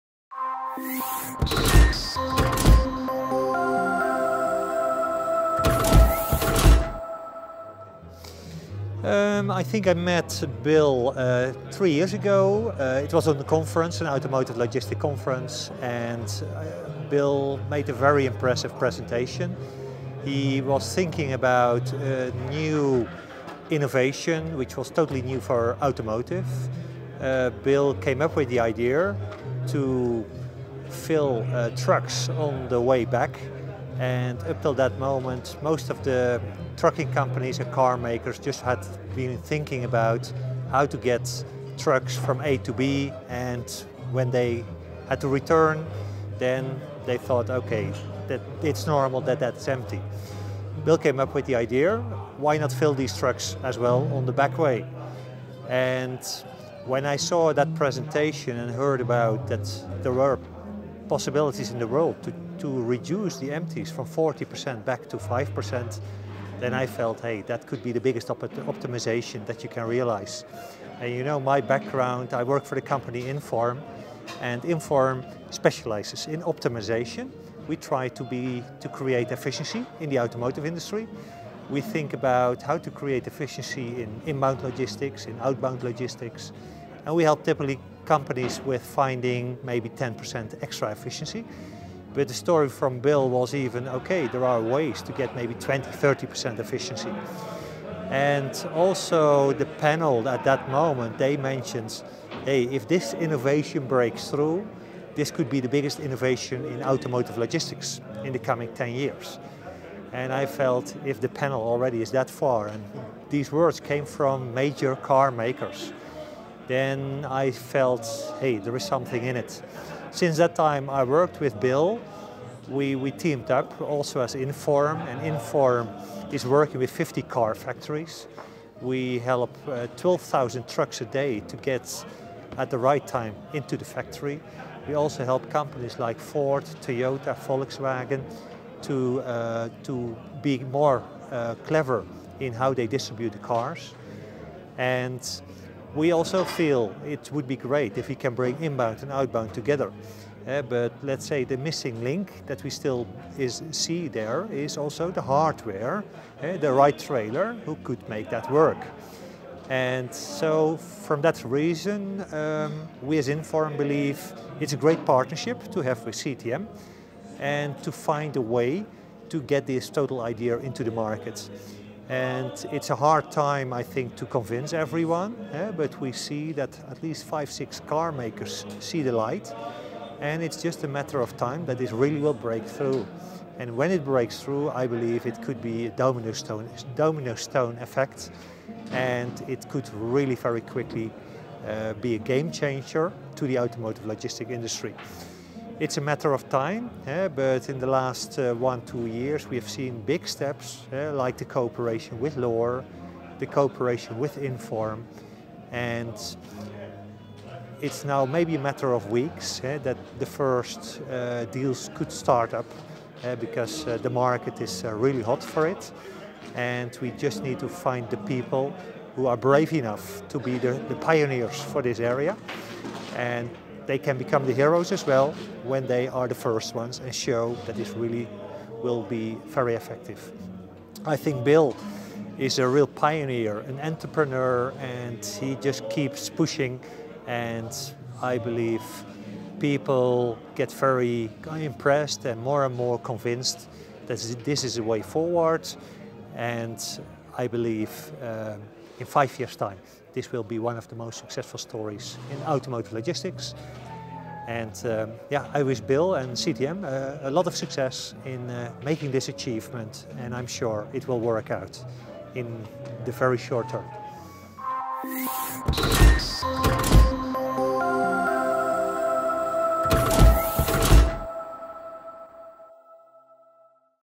The Convertible Trailer with Ruud Vossebeld
Ruud Vossebeld, INFORM, speaks about the value of the Convertible Trailer and the effect it will have on the entire industry.